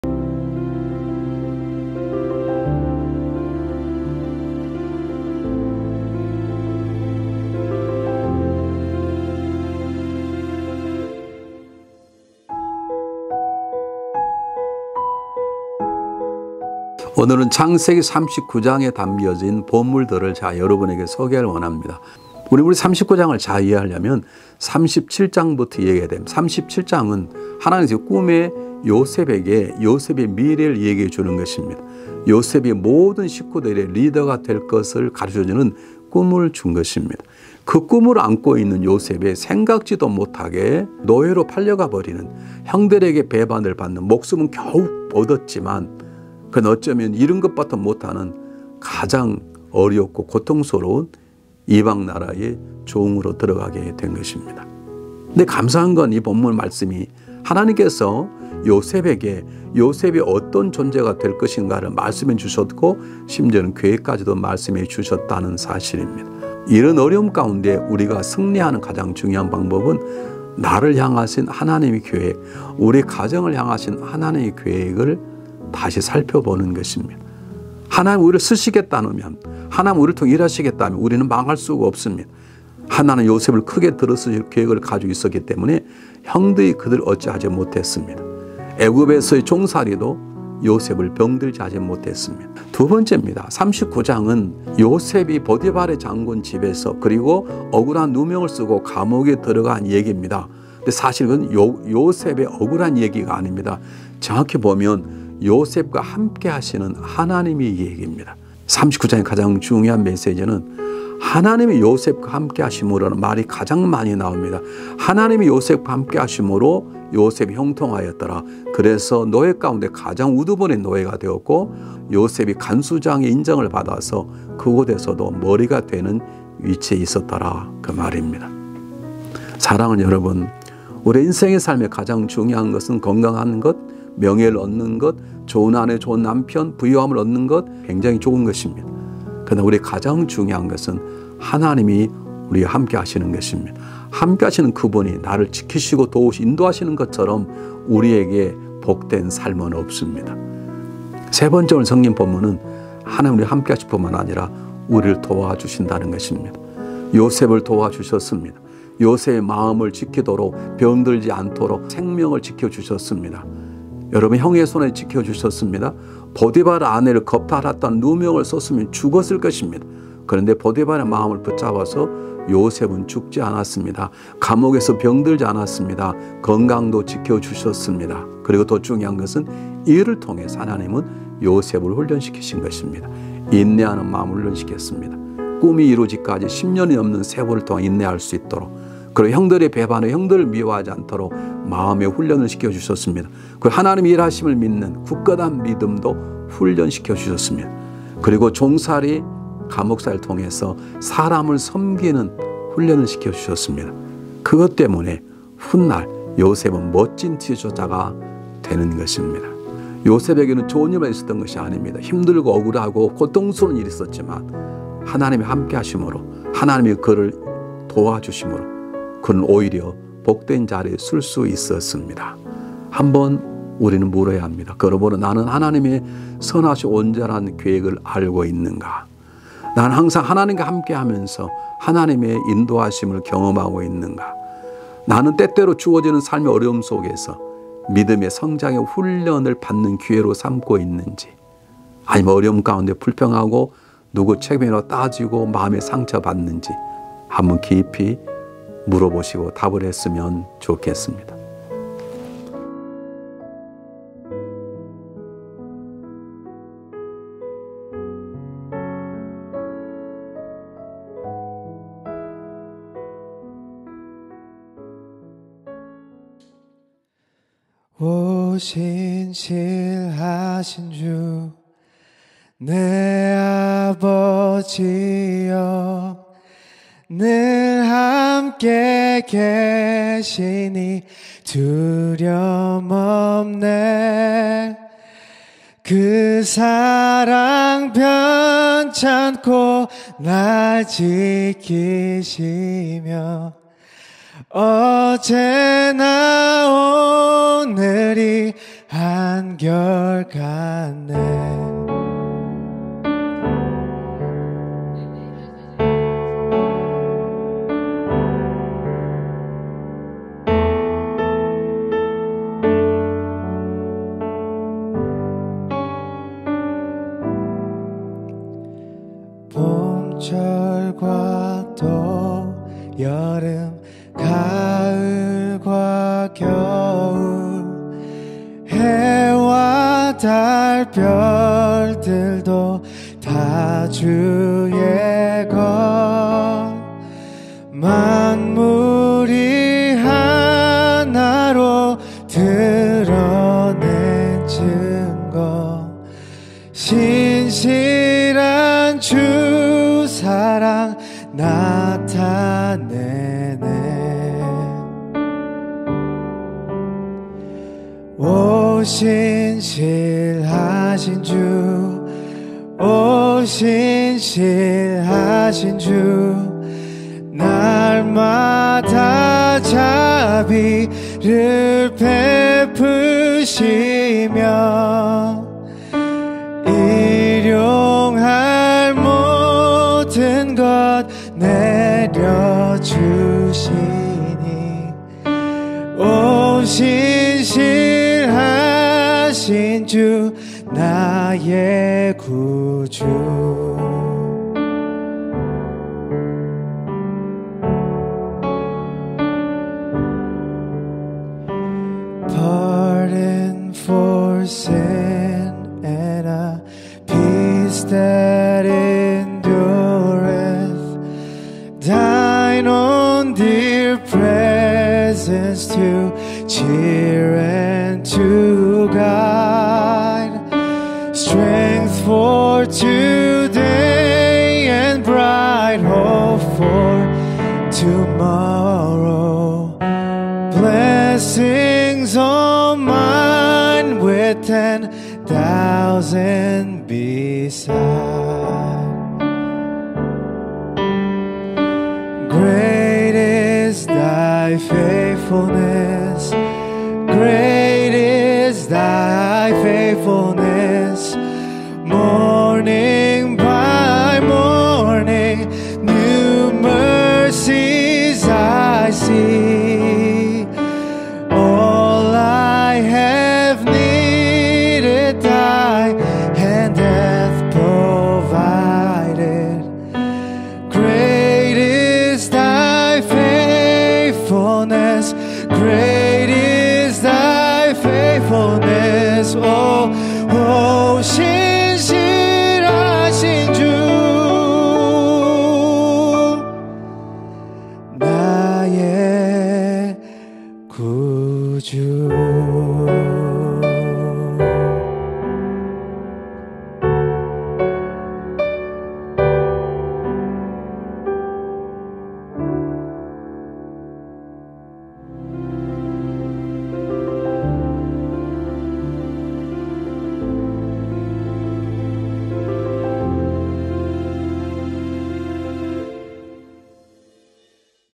'창세기 안에 담겨진 보물' 열번째 이야기 2021.6.28
이성철 담임 목사님의 SC 성경이야기
 '창세기 안에 담겨진 보물'

찬양 - 김진영 형제
건반 - 김현지 자매

매주 월요일 오전 10시 업로드 됩니다.

배경음악
크리스찬 BGM